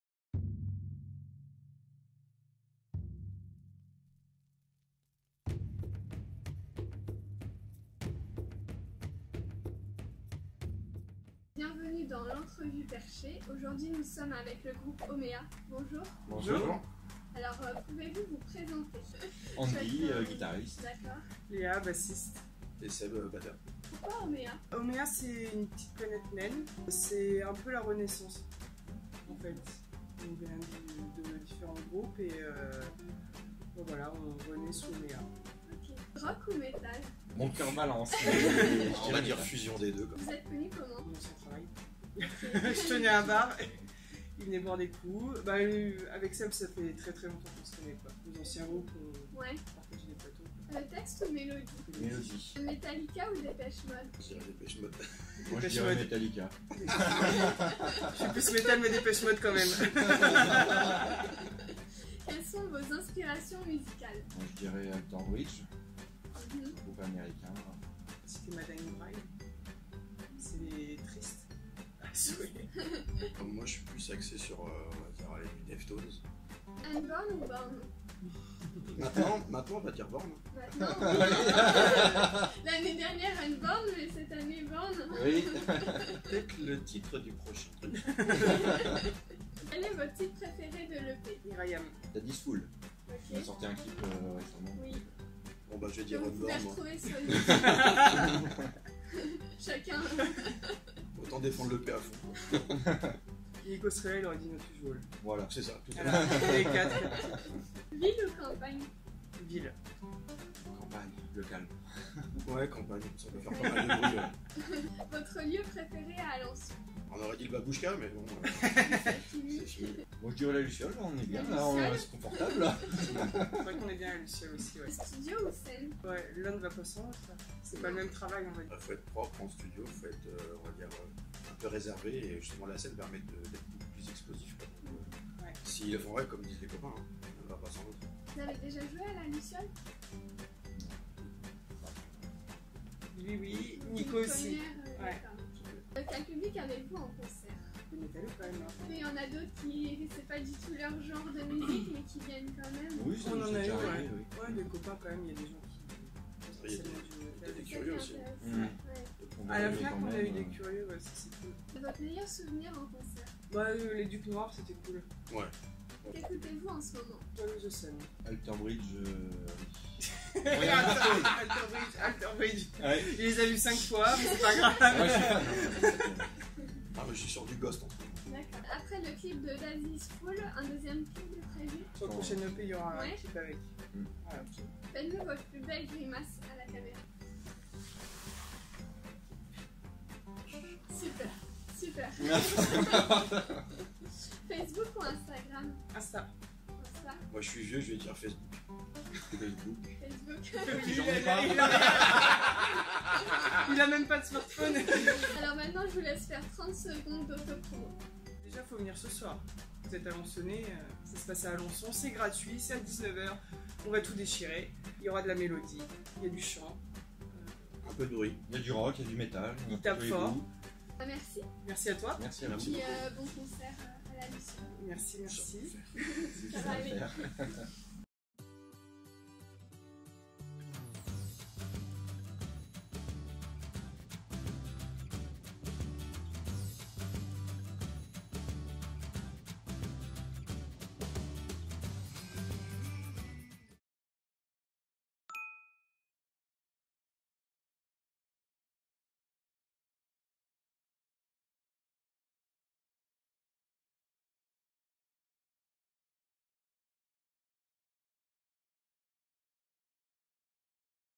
L'Entrevue Perchée #54 - Hauméa
Un smartphone, une perche à selfie, un artiste ... C'est l'Entrevue Perchée !

A l'occasion de leur concert à La Luciole, à Alençon, nous avons  rencontré le groupe Hauméa pour leur poser quelques questions !

Merci à Hauméa pour l'interview.

- 31/05/2019 La chapelle St Aubin (Cosmozik fest)
- 14/06/2019 La Ferrière Bochard (Le Source fest)
- 10/08/2019 Grand Champ (Les Libres Music Halles)
- 30/08/2019 Cossé en Champagne (Keudfest)